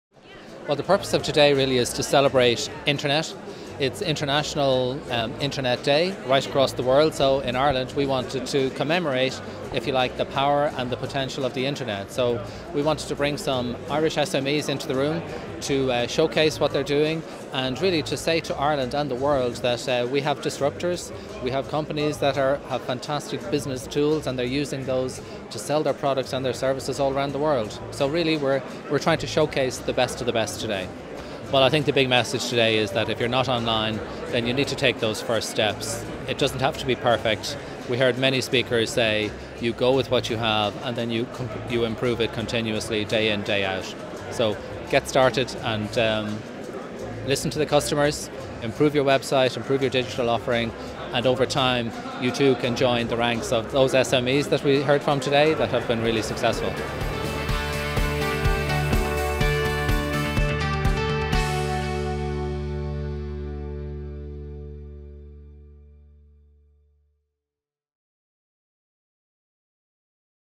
IEDR CEO David Curtin at Ireland's Internet Day 2016
IE Domain Registry hosted an event to mark Ireland's Internet Day at the Mansion House in Dublin, on 27 October 2016.